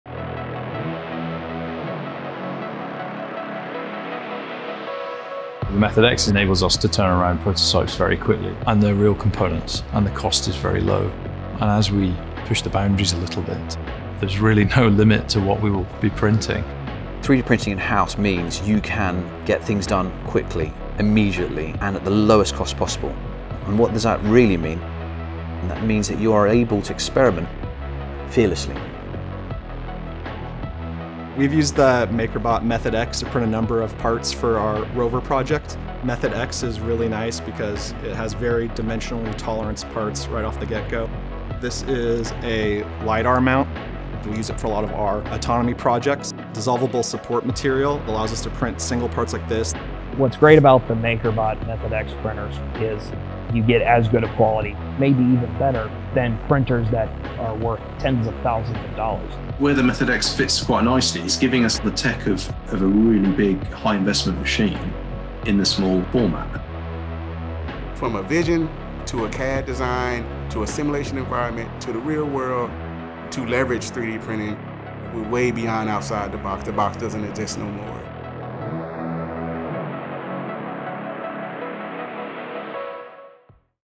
METHOD PRO Customer Highlights 2021
2021 was an exciting year for MakerBot PRO customers and we got the opportunity to work with several talented designers and engineers in the automotive and aerospace industries.

These are a few select highlights of our METHOD X customers who printed everything from concept models to working carbon fiber car parts!

Special thanks to:
Lockheed Martin
Prodrive
CALLUM
Spire Motorsports
Arash Motor Company

Watch the story on MakerBot’s Aerospace Pro Customer Lockheed Martin - 3D printing parts with METHOD X for Project Artemis